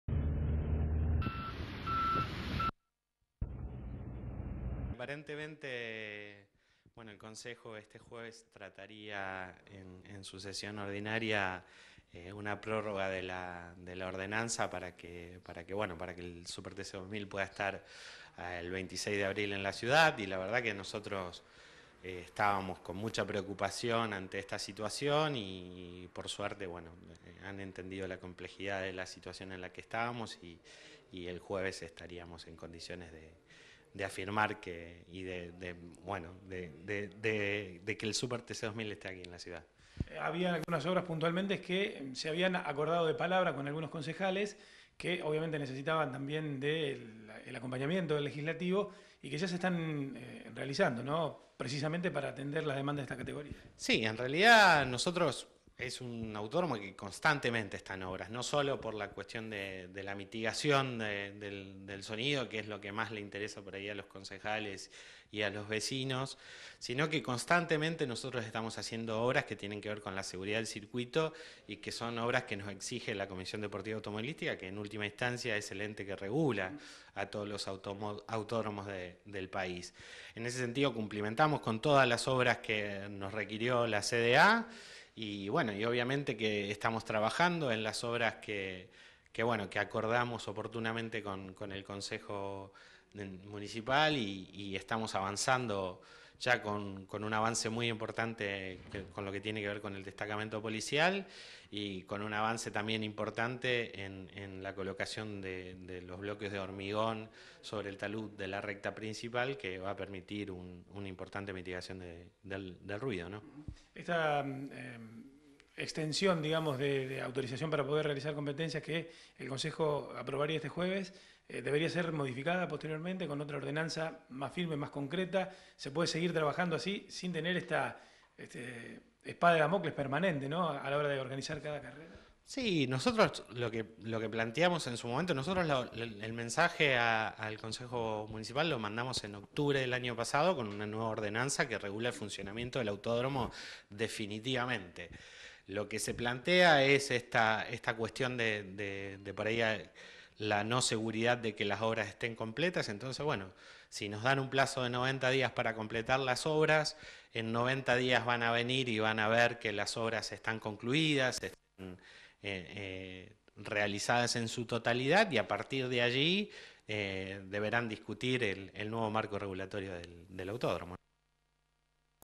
Realizan obras en el Autódromo Municipal para llegar a la fecha del TC 2000.
CONSTRUYEN MURALLA PARA MITIGAR EL SONIDO
MAÑANA SE TRATA EL TEMA EN EL CONCEJO
MAURICIO MALANO – DIRECTOR DEL AUTÓDROMO

En el calendario oficial del Super TC 2000, Rosario está confirmada como sede de la 2da fecha del año 2015, que se realizará el 26 de abril. El en lugar se están haciendo obras de insonorización y una garita policial. El Concejo Municipal aún no decidió su aprobación, pero confían que mañana se tratará el tema.